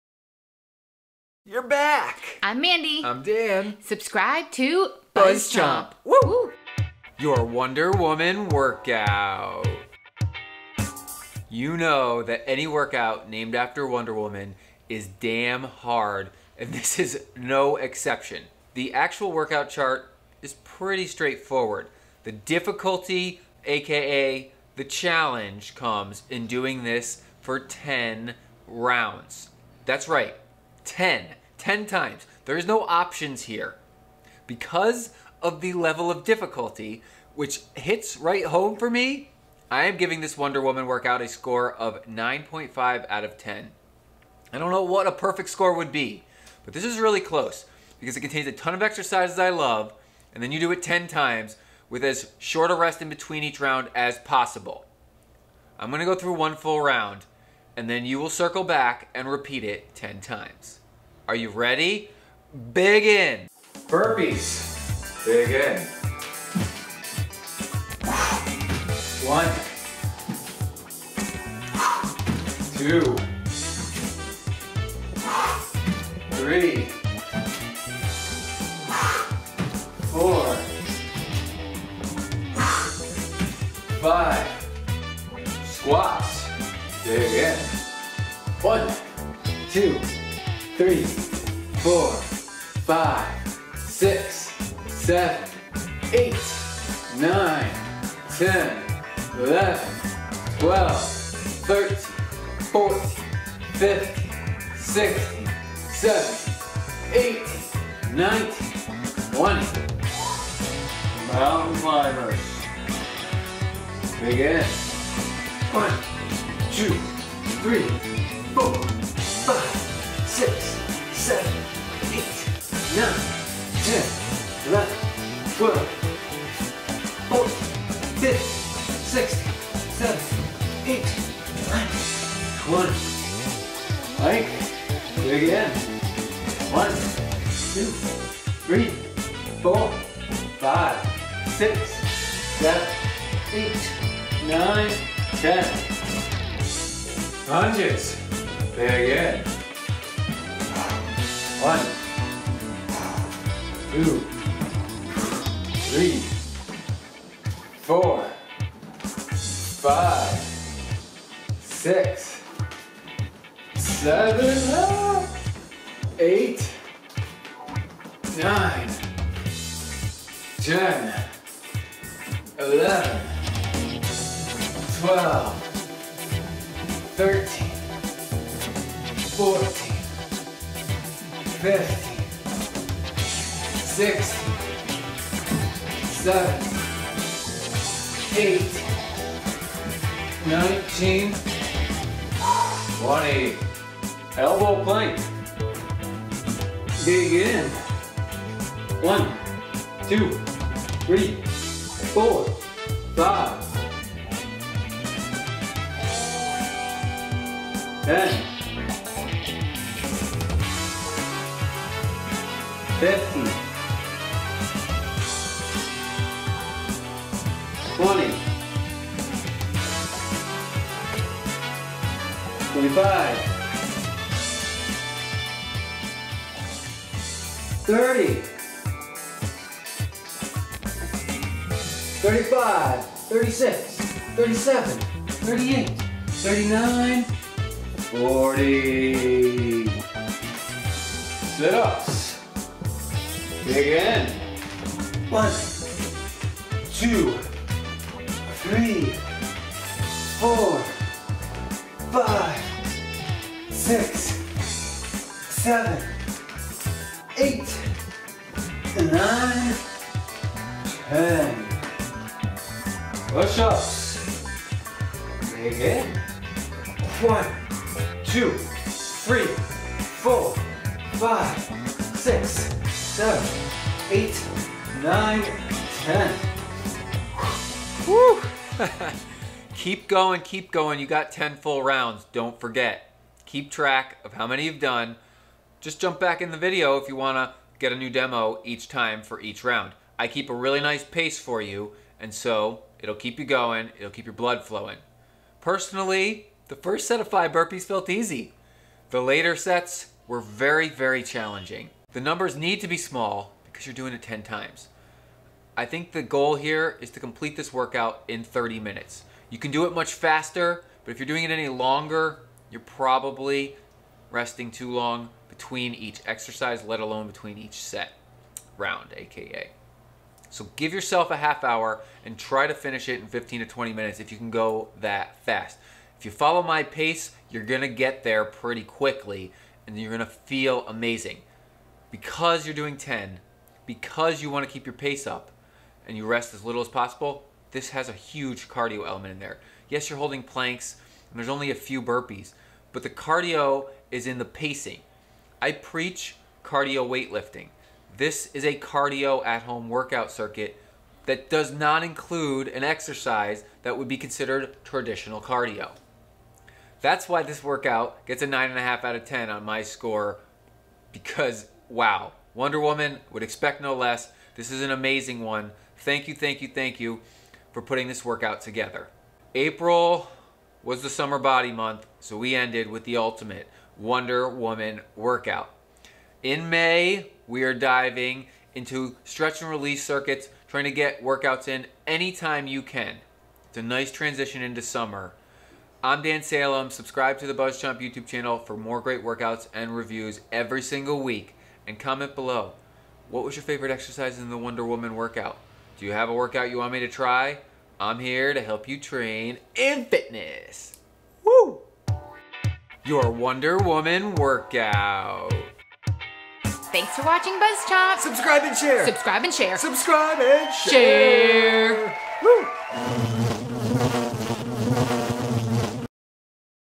Wonder Woman Workout Video and Review | BuzzChomp Fitness
The Wonder Woman workout gets our highest rating so far at a 9.5/10. It is 8 exercises that I have done more times than I can count. These exercises anyone can do and that's really the magic of the Wonder Woman workout that at first glance it is not difficult. Now repeat it 10 times and yes that is right I said 10 times!

Repeating the Wonder Woman workout is confronting, fun, and works your body from head to toe. I know what you're thinking that you can't do this circuit 10 times, but you can with a solid pace. None of the reps for the exercises are extreme and that lends it to holding a steady pace.

A big tip here to getting the most benefits out of the Wonder Woman workout is to keep with the two minutes rest between rounds. If you're very physically agile then you could rest for a minute, but you don't want to burn out. That being said don't go too aggressive with your pace because you will burn out.

I love body weight workouts and circuits that you can do at home that push me to my limits. This Wonder Woman workout does that and it does it because it takes simple exercises, at low reps, and makes you repeat them 10 times! If you repeat say burpees 10 times you're going to feel, but now add in everything to follow.

If you're a sweat junky like me or if you're just getting into working out, you will appreciate this Wonder Woman workout! Try it out and let me know how you felt after 10 rounds!